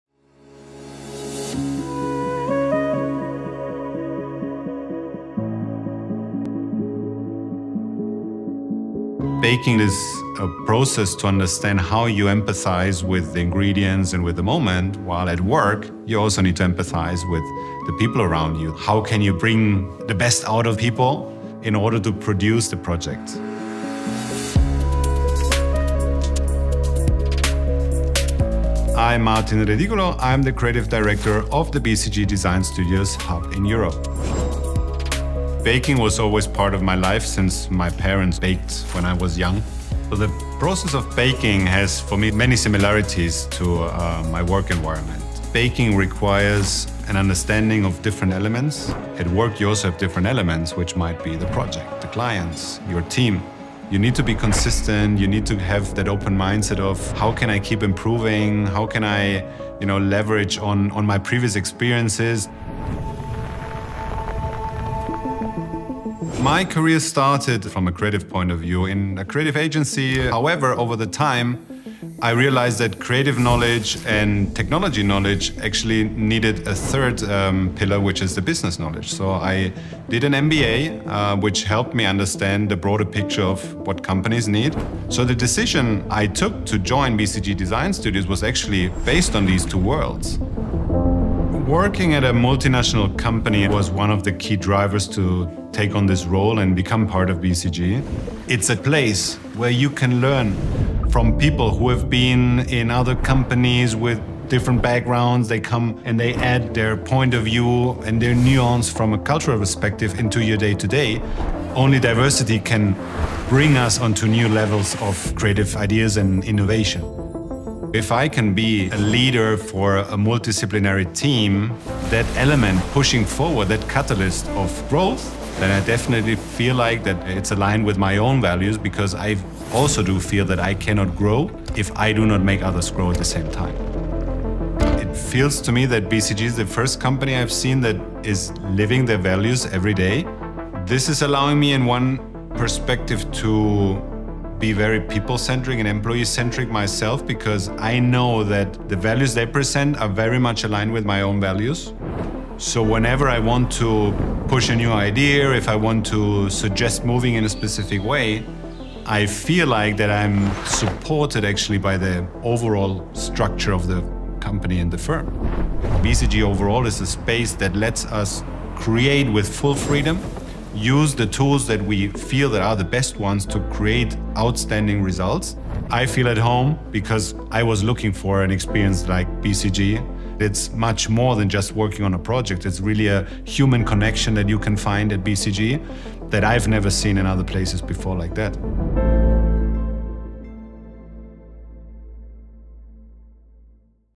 Meet Martin: Benefitting from a Growth Mindset
Baking, much like consulting, requires a growth mindset, finds Martin Redigolo, Creative Director of BCG Design Studios - Madrid. "Baking requires an understanding of different elements. At work we have different elements that might be the project, the client, or your team. You need to be consistent, have that open mindset, and [ask] how can I keep improving," he says.

In his career, Martin looks to bridge together the two worlds of creativity and business, and found that a role at BCG Design Studios, BCG's in-house global creative agency, would allow him to do so. Being at BCG also allows Martin to be a part of diverse teams at work. He has found there are many benefits of a diverse team, saying "Only diversity can bring us onto new levels of creative ideas and innovation."

Martin values the opportunity BCG gives him to bring the best out of people on his team and build human connection at work. "I feel at home because I was looking for an experience like BCG. It's much more than working on a project," he says.